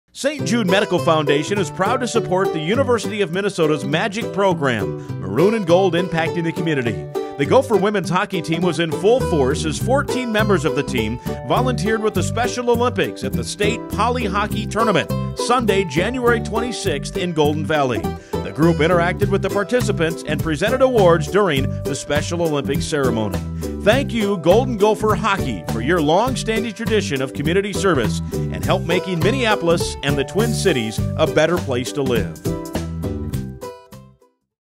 Gopher Women's Hockey Volunteers for Special Olympics
On Jan. 26, 2014, members of the Golden Gopher women's hockey team volunteered with the Special Olympics at the Minnesota state poly hockey tournament.